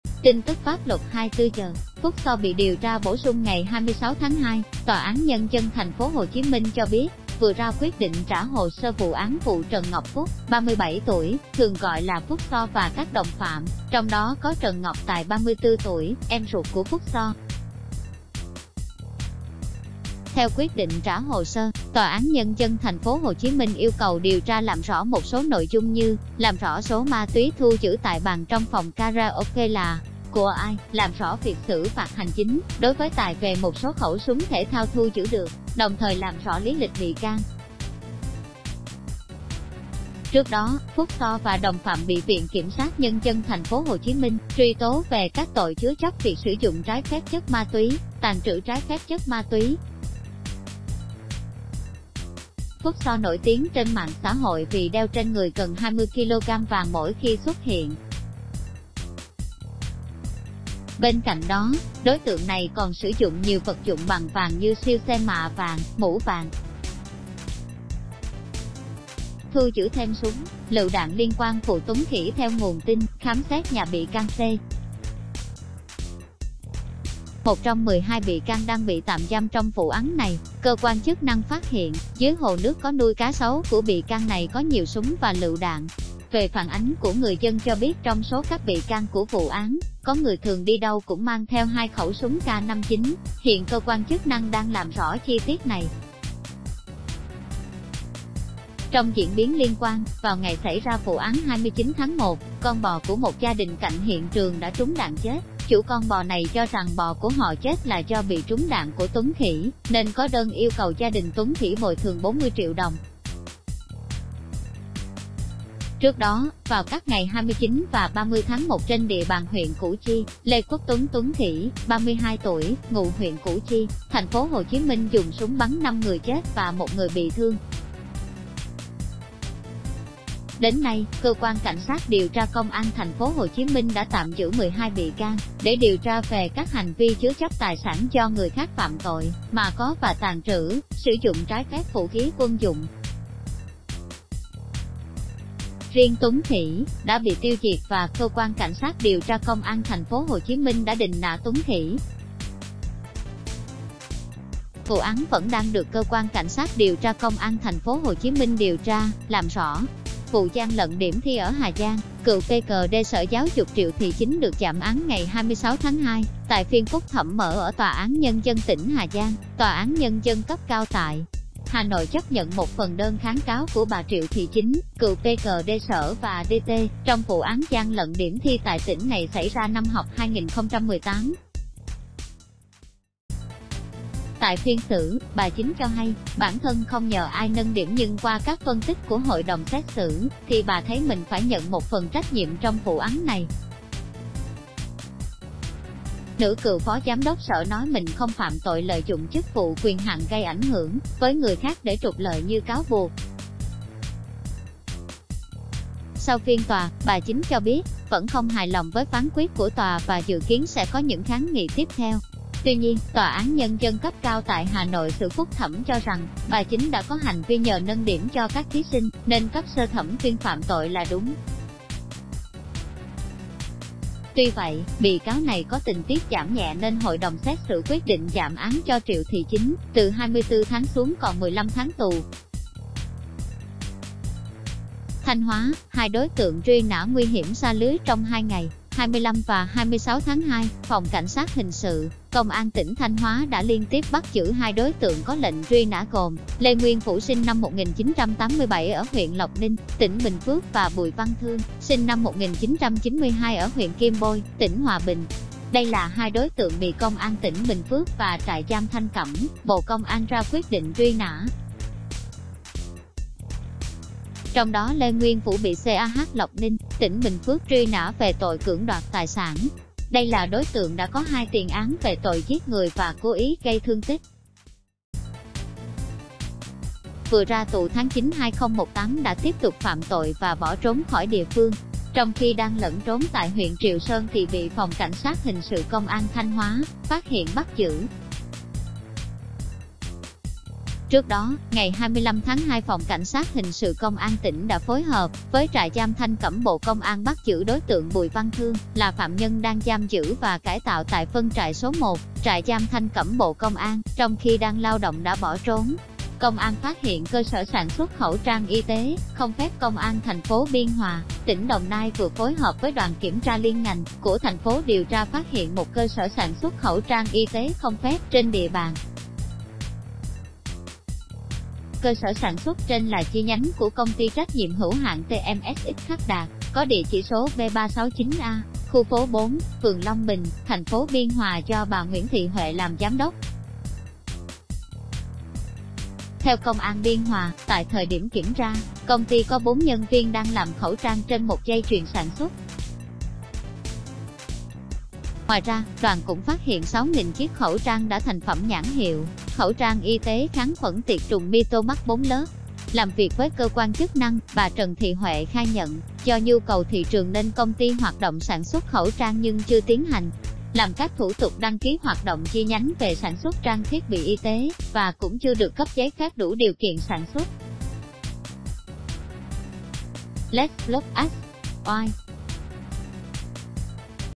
Tin tức pháp luật 24h: Phúc XO bị 'sờ gáy', phát hiện mới vụ Tuấn 'khỉ'
Tin tức pháp luật 24h: Phúc XO bị điều tra bổ sung Ngày 26/2, TAND TPHCM cho biết, vừa ra quyết định trả hồ sơ vụ án vụ Trần Ngọc Phúc (37 tuổi, thường gọi là Phúc XO) và các đồng phạm, trong đó có Trần Ngọc Tài (34 tuổi, em ruột của Phúc XO). Theo quyết định trả hồ sơ, TAND TPHCM yêu cầu điều tra làm rõ một số nội dung như: làm rõ số ma túy thu giữ tại bàn trong phòng karaoke là của ai, làm rõ việc xử phạt hành chính đối với Tài về một số khẩu súng thể thao thu giữ được, đồng thời làm rõ lý lịch bị can. Trước đó, Phúc XO và đồng phạm bị VKSND TPHCM truy tố về các tội "Chứa chấp việc sử dụng trái phép chất ma túy", "Tàng trữ trái phép chất ma túy". Phúc XO nổi tiếng trên mạng xã hội vì đeo trên người gần 20 kg vàng mỗi khi xuất hiện. Bên cạnh đó, đối tượng này còn sử dụng nhiều vật dụng bằng vàng như siêu xe mạ vàng, mũ vàng. Thu giữ thêm súng, lựu đạn liên quan vụ Tuấn 'Khỉ' Theo nguồn tin, khám xét nhà bị can C. (1 trong 12 bị can đang bị tạm giam trong vụ án này), cơ quan chức nă...